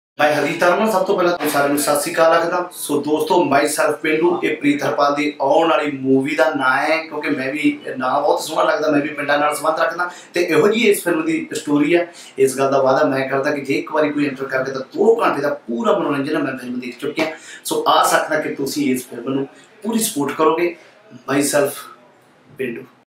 Harjit Harman | Preet Harpal's Movie - MySelf Pendu | Director - Surinder Rihal
Best Wishes for Myself Pendu Film from Harjit Harman.
Starring: Preet Harpal, Sayali Bhagat, Ather Habib, Jaswinder Bhalla, Upasna Singh, Satinder Satii & Surinder Rihal.

Directed By: Surinder Rihal.

Produced By : Music Box Media.